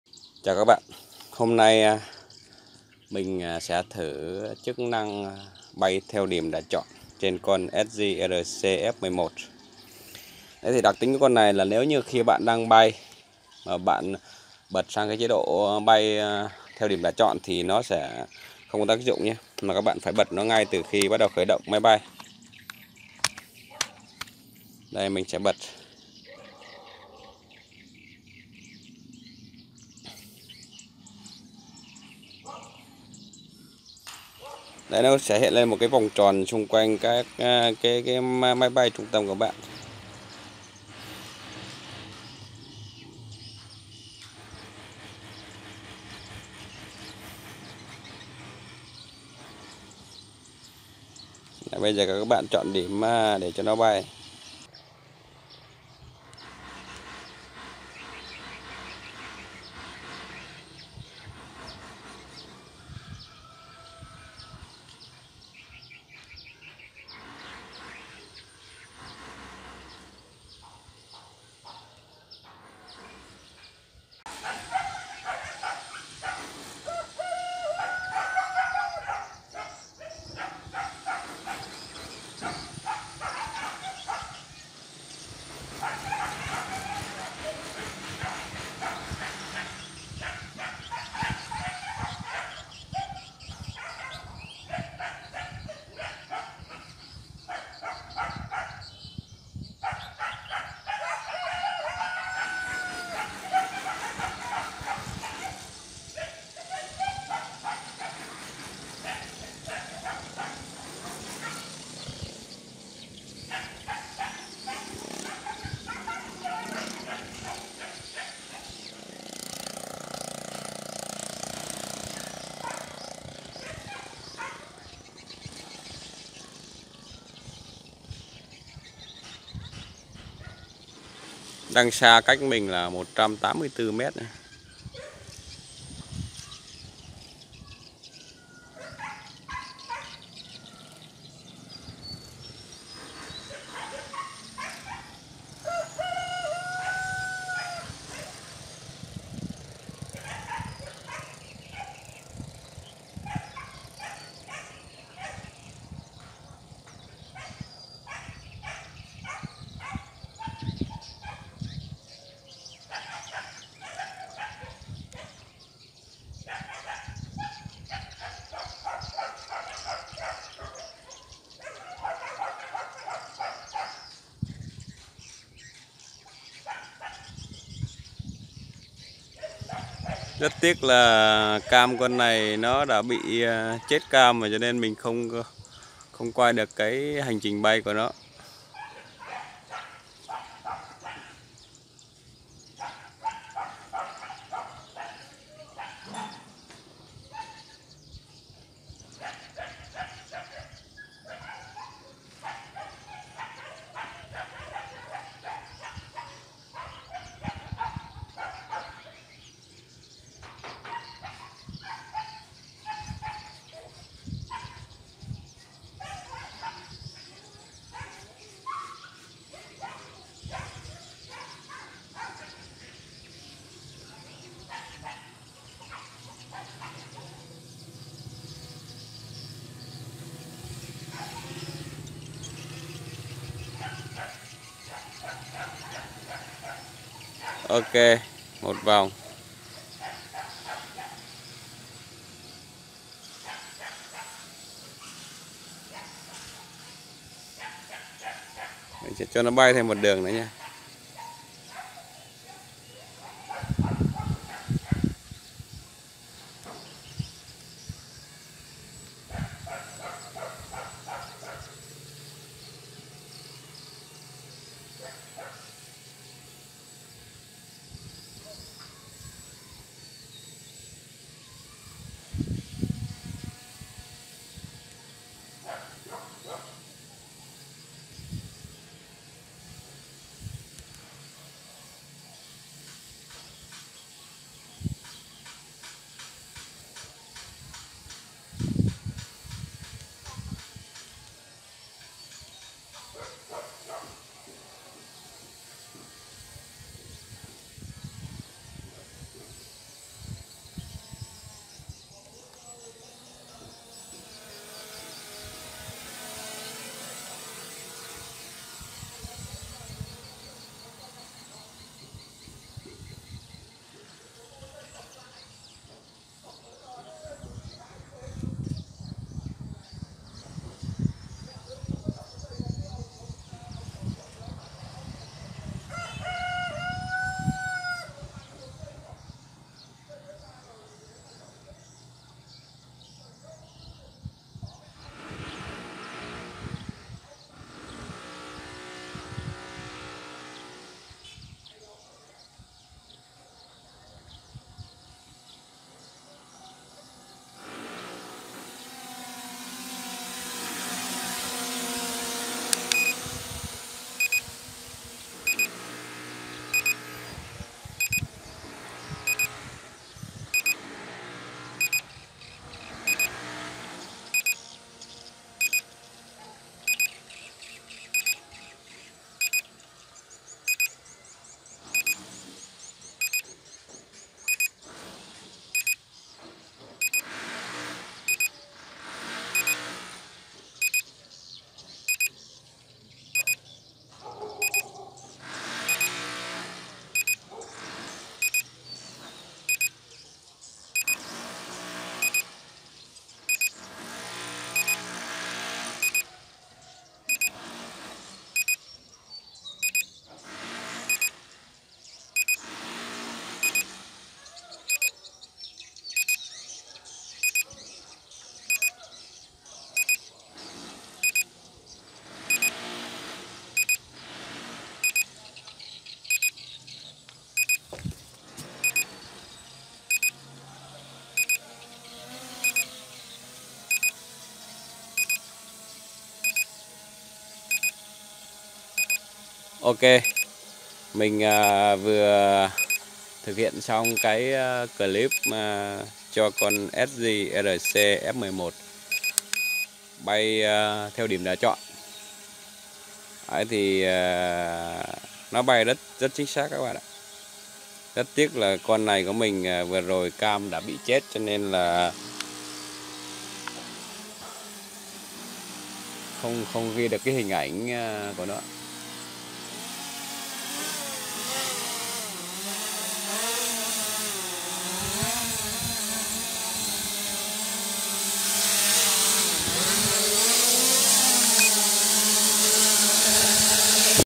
SJRC F11 tính năng bay theo điểm đã chọn
Tính năng bay theo điểm đã chọn trên SJRC F11. Để sử dụng tính năng này các bạn phải khởi động nó khi may bay chưa cất cánh nhé. Nêu máy bay đang bay rồi thì bạn khởi động chức năng nay sẽ bi văng App. Tính năng bay theo điểm đã chọn trên SJRC F11 mình tét thử có thể bay được 7 điểm.